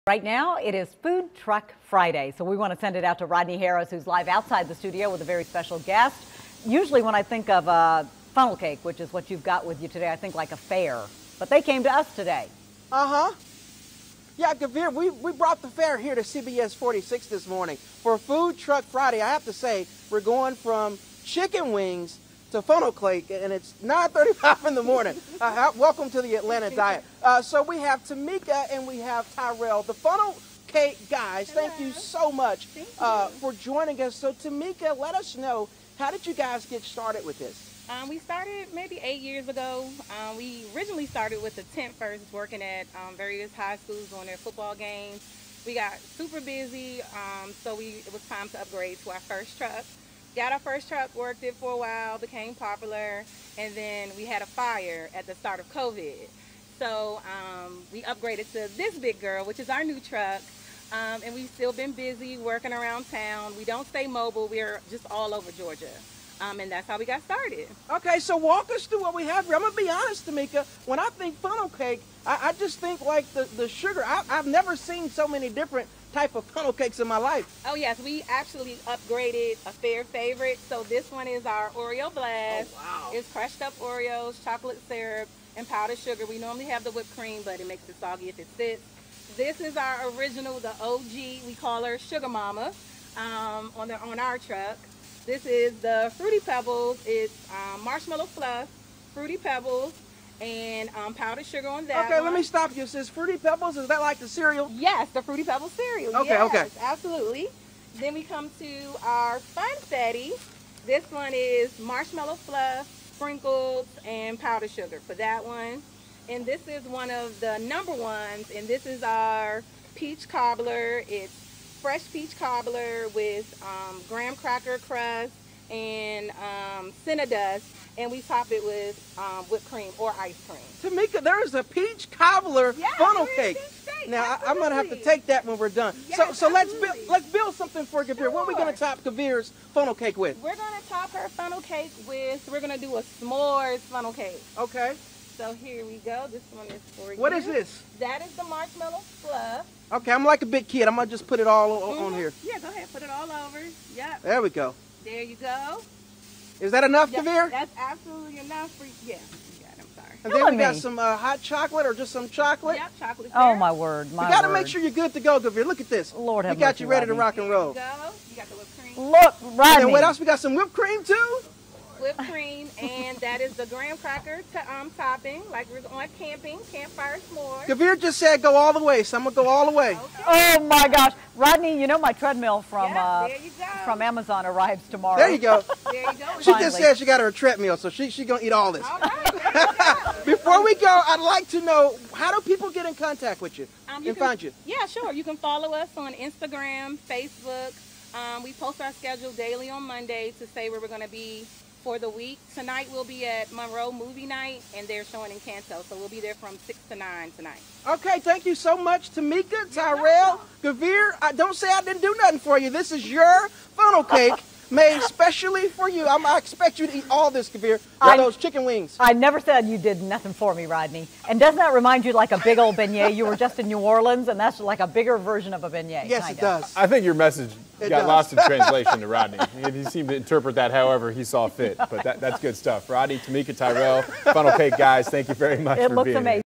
Food Truck Friday: The Funnel Cake Guys stop by CBS 46!
The Funnel Cake Guys stopped by CBS 46 to show off their best creations for FoodTruckFriday. Full story